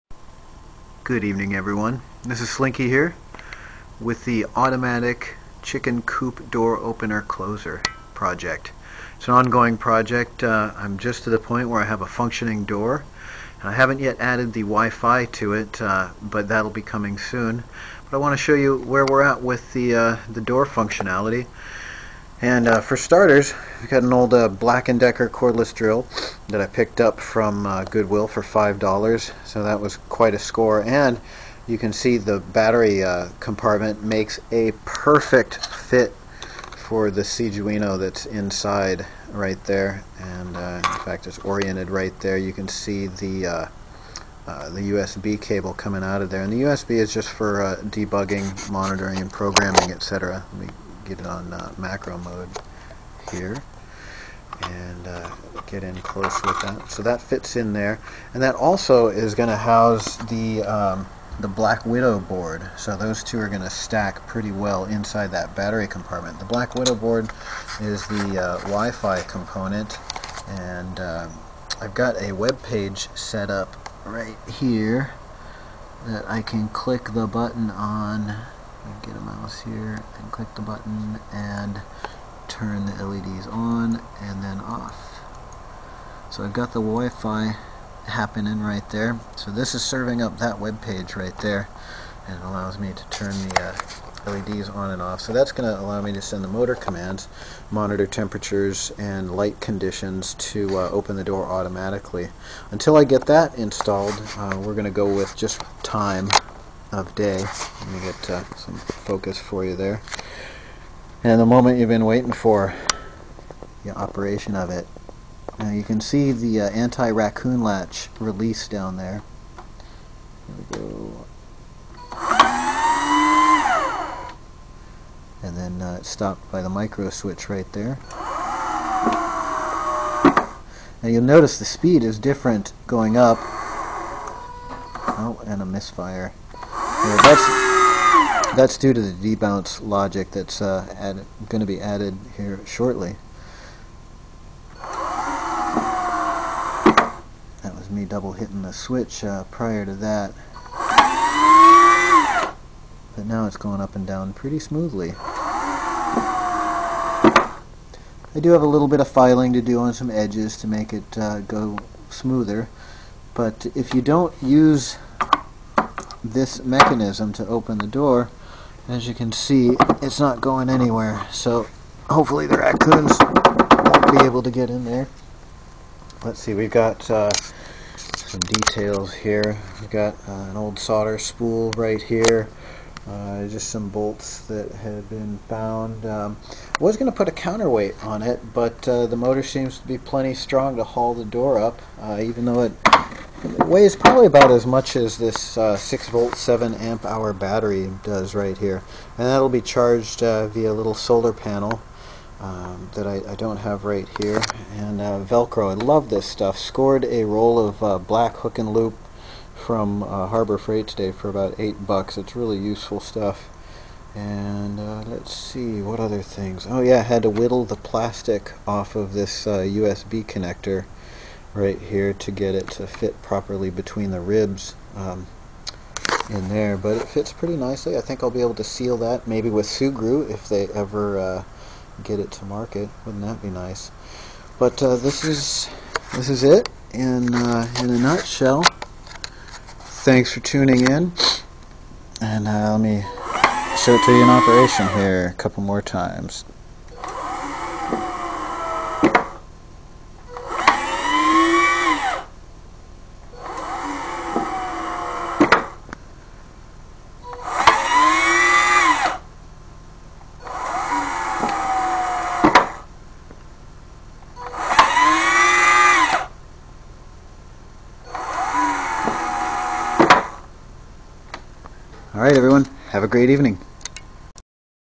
Wireless Door Opener - Electrical Final Year Engineering Project Ideas.flv
final year projects by united institute of technology college student(uit) gaurav khare
 branch en